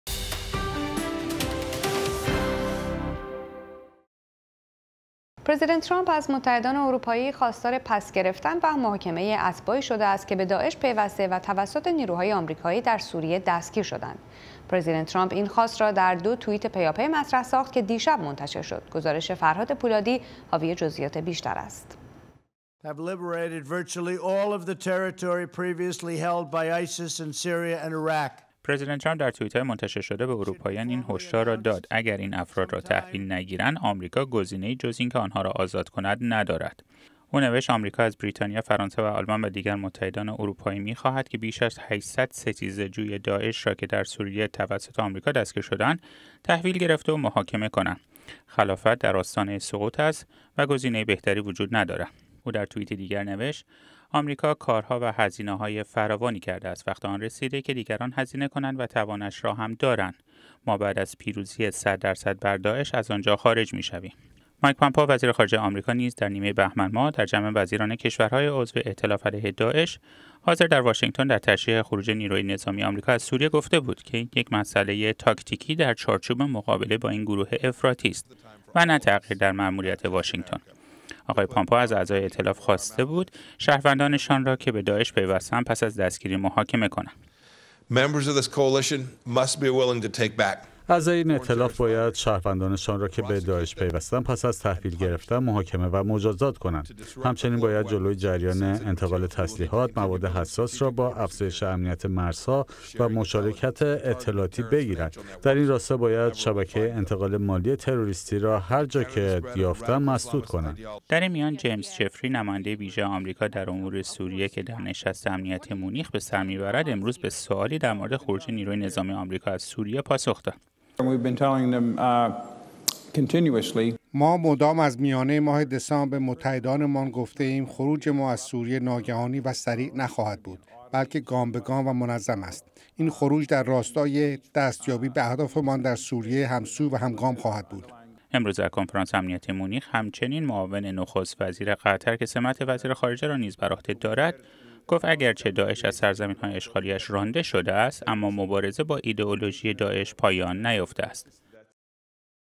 پرزیدنت ترامپ از کشورهای اروپایی خواست اتباعشان را که به داعش پیوسته بودند، تحویل بگیرند
پرزیدنت ترامپ از متحدان اروپایی آمریکا خواست شهروندانشان را که به داعش پیوسته بودند و توسط نیروهای آمریکایی در سوریه و عراق بازداشت شده اند، تحویل گرفته و محاکمه کنند.